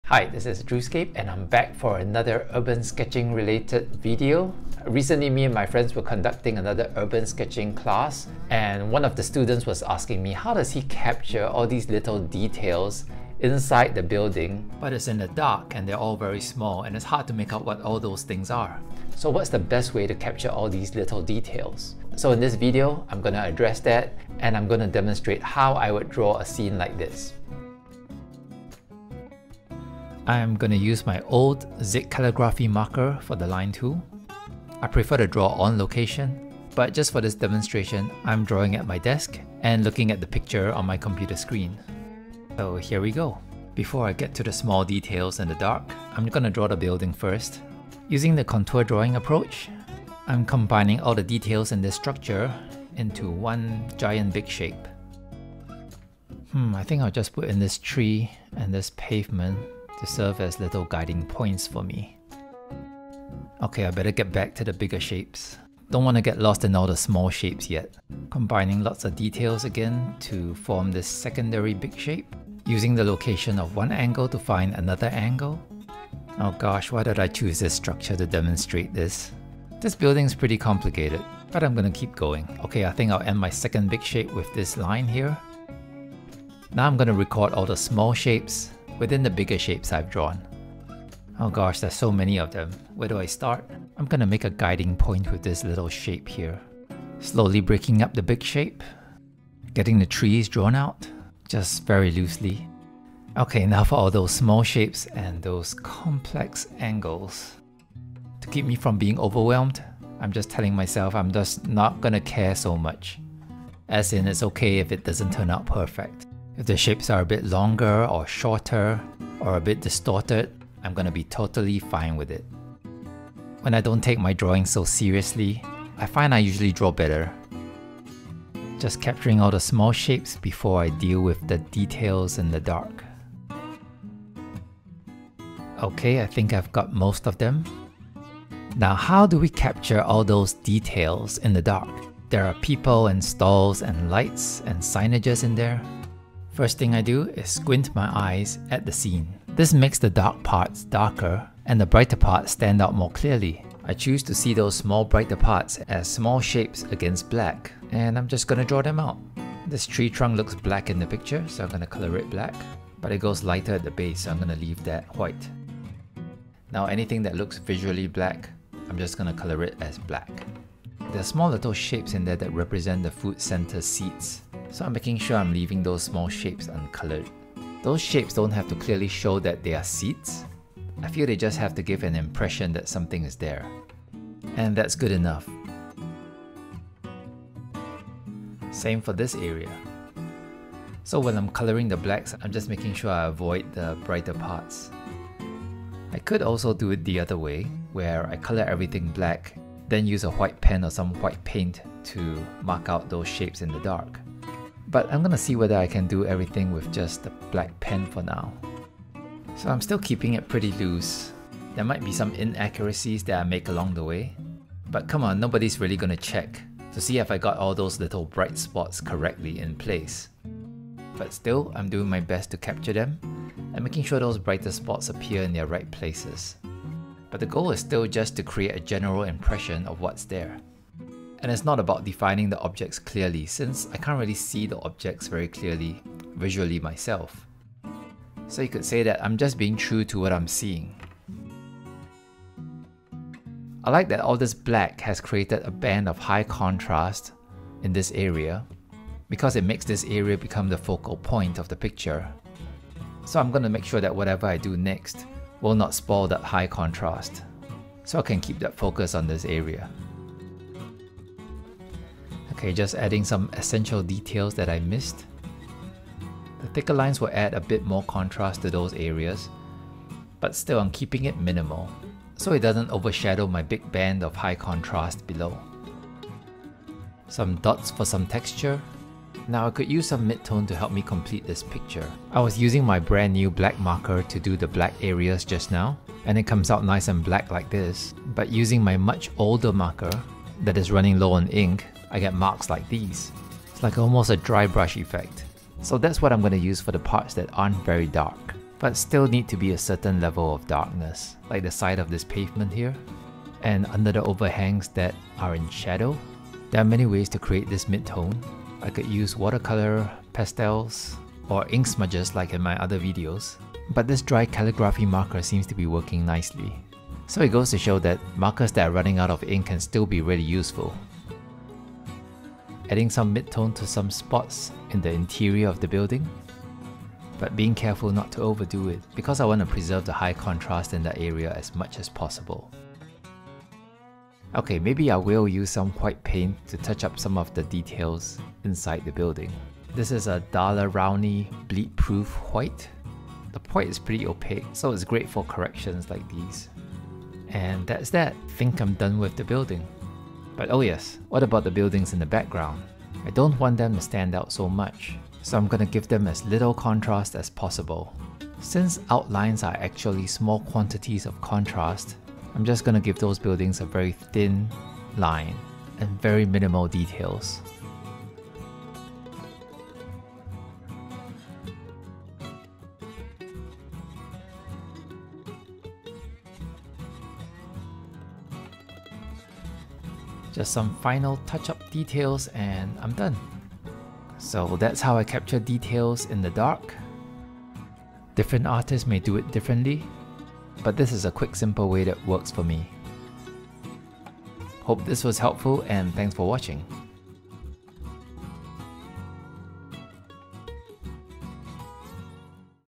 How to sketch small details in the dark
Drawing small details in the shade, like within shops, and eateries can seem like a daunting task because it's hard to make out what they are in the dark and they also appear so small. This is one quick way I use to capture them.

(I'm gonna work on producing more of these down the road! If you've already purchased them, thank you!)

Want to draw quicker, smaller and simpler?